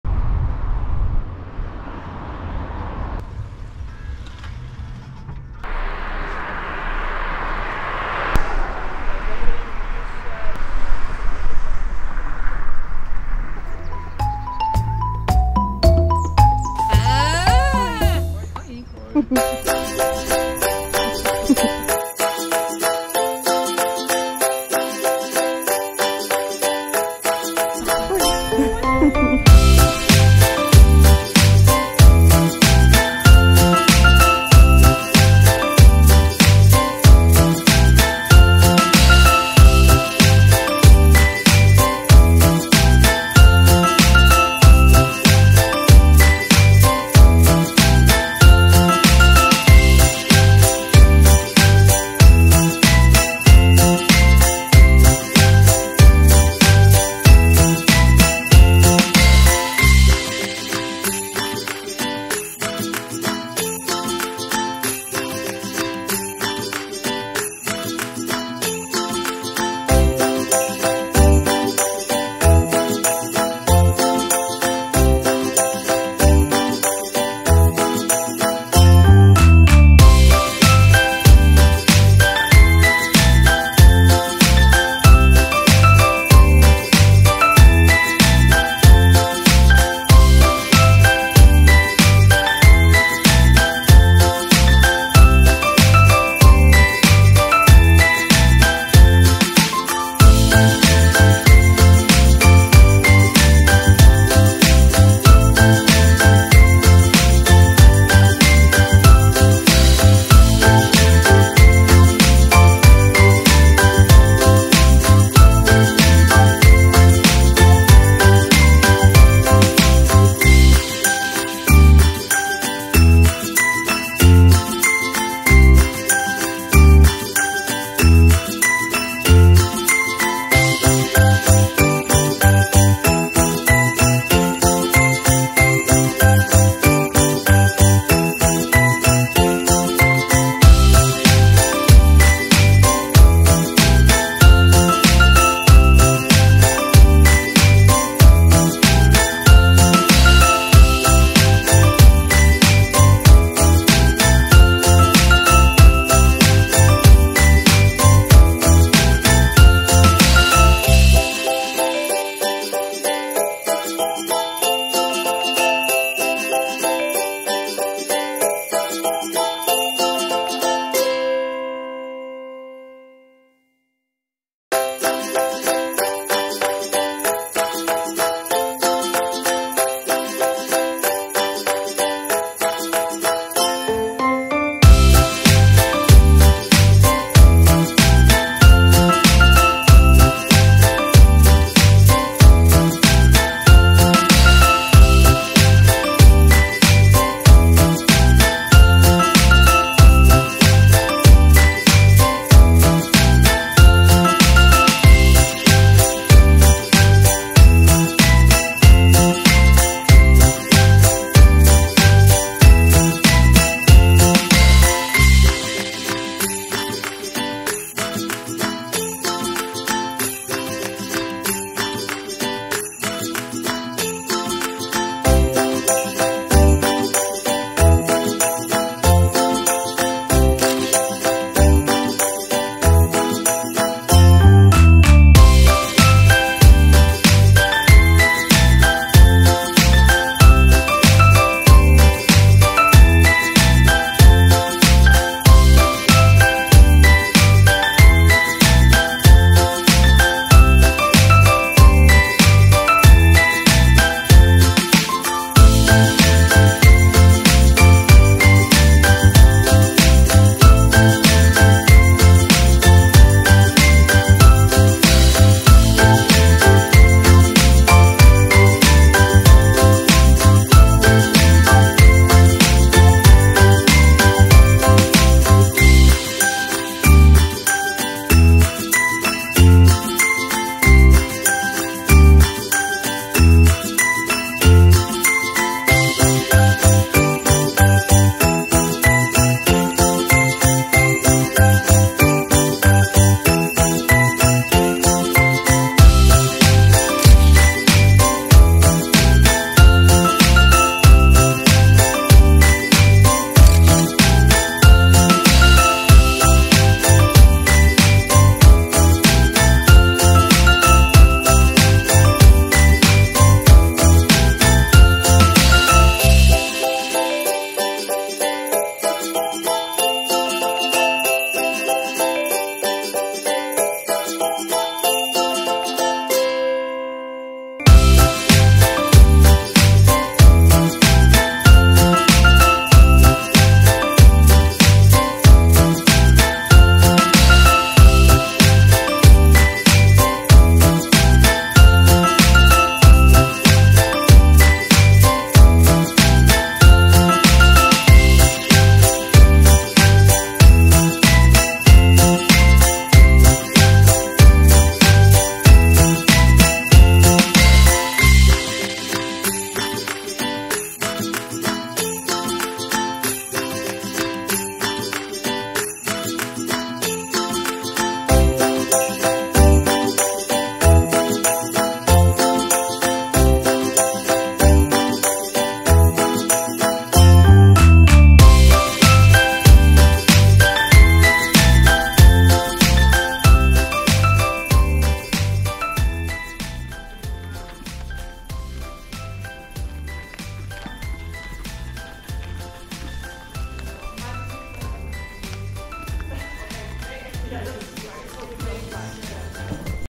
Oliebollendag bij Alpacas Zeelandia
Alpacas Zeelandia heeft een geweldig leuke dag georganiseerd met allerlei leuke activiteiten met Alpaca's.

Help jij ons alpaca's in nood helpen?
Steun Stichting Alpaca Rescue Nederland.

Wil je een wandeling maken met onze alpaca's of wil je bij ze op de koffie of high tea? De opbrengst van alle activiteiten gaat naar onze rescue alpaca's.

Gebruikte apparatuur:
- GoPro Hero 10, Sony A7Rii